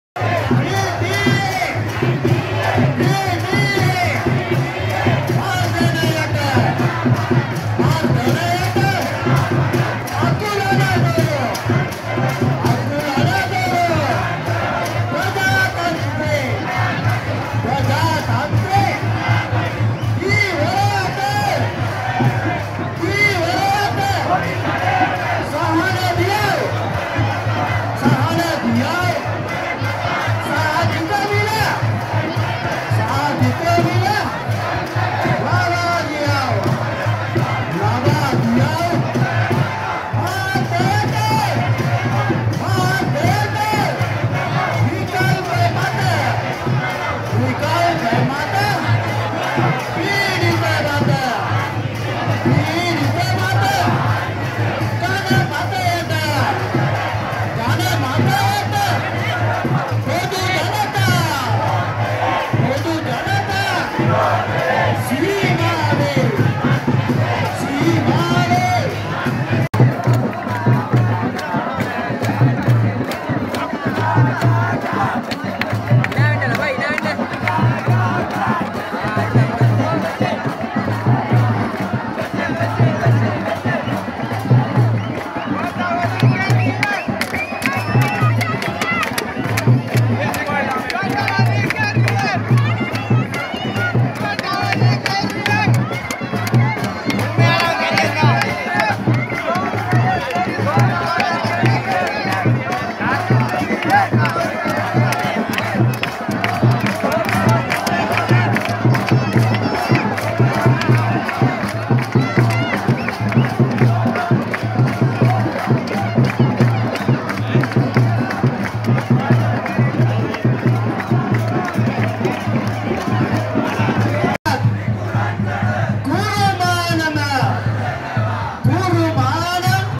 Seethawakapura FTZ workers go on strike & protest urging President Gotabaya to step down
Seethawakapura free trade zone workers go on strike & protest urging GotaGoHome on 6th May